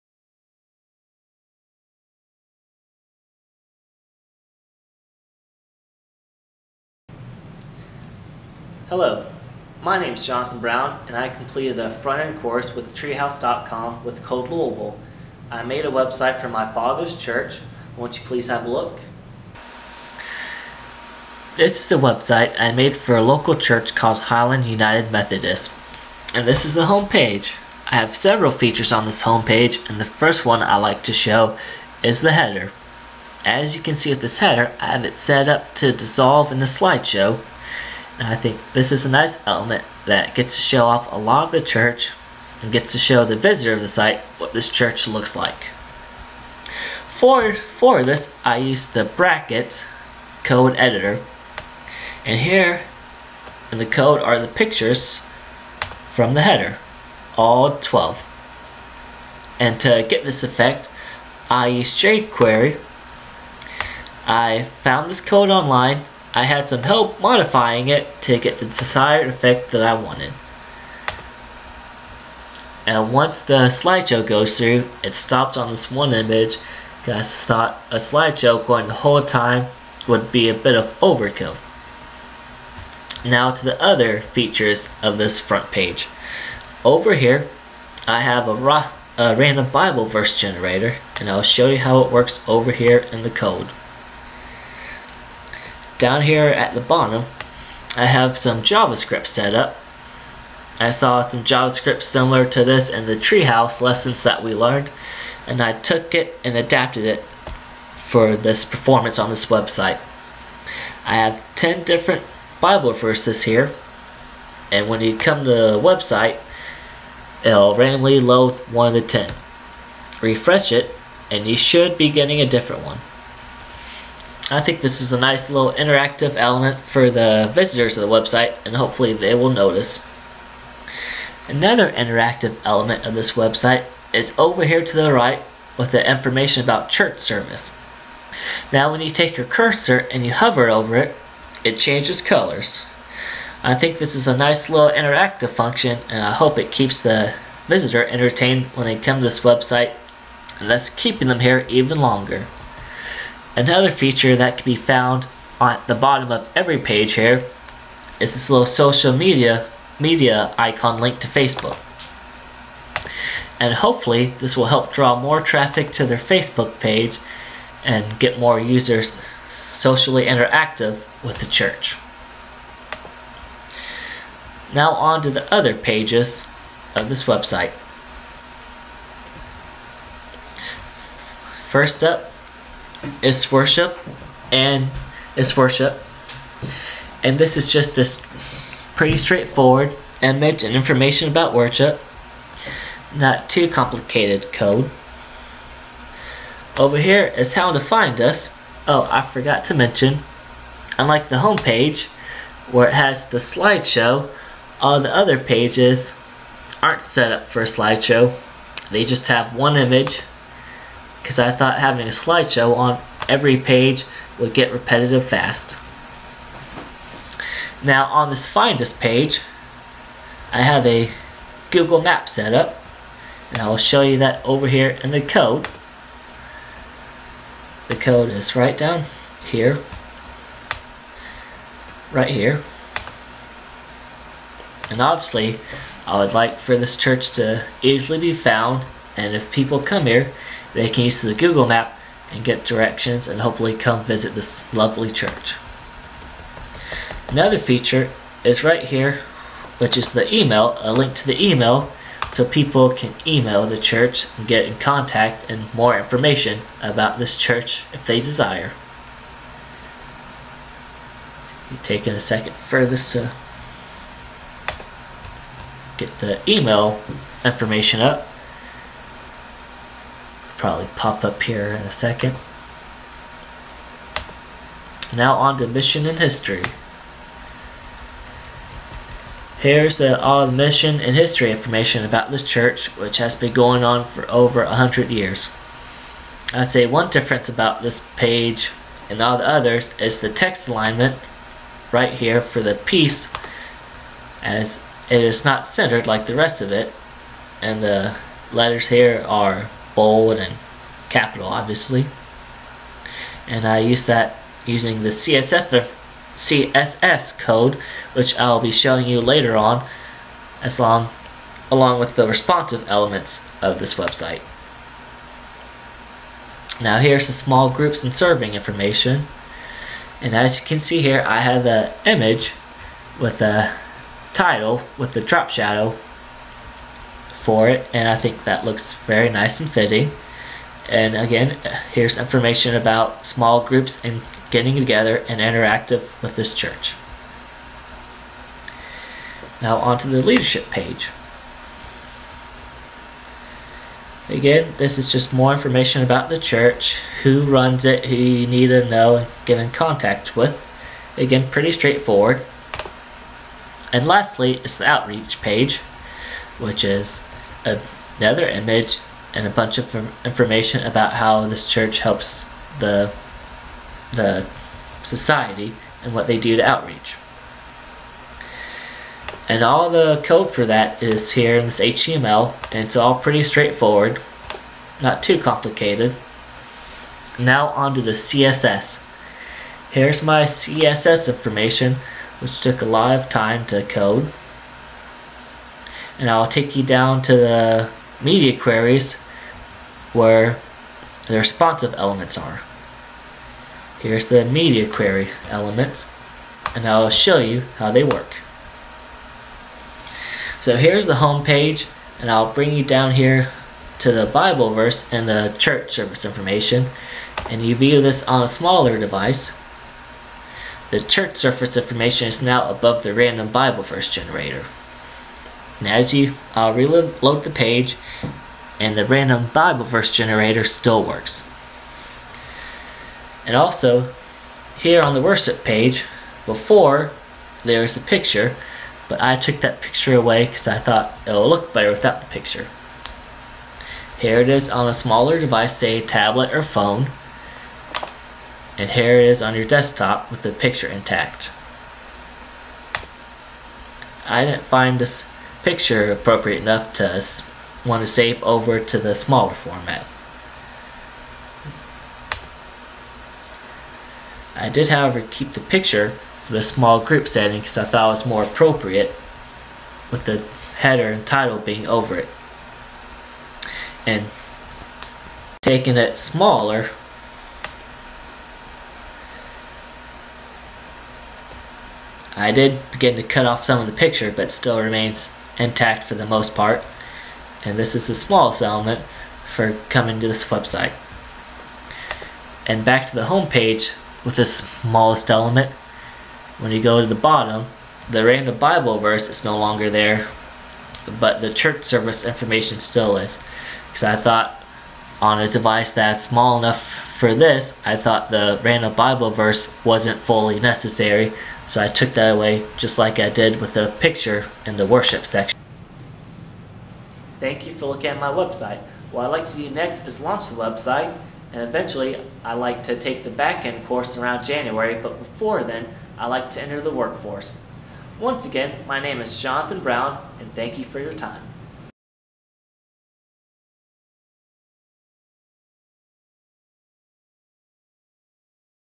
Jonathan Brown (Front End Web Development)
Meet Jonathan Brown, a graduate of Code Louisville's Front End Web Development track!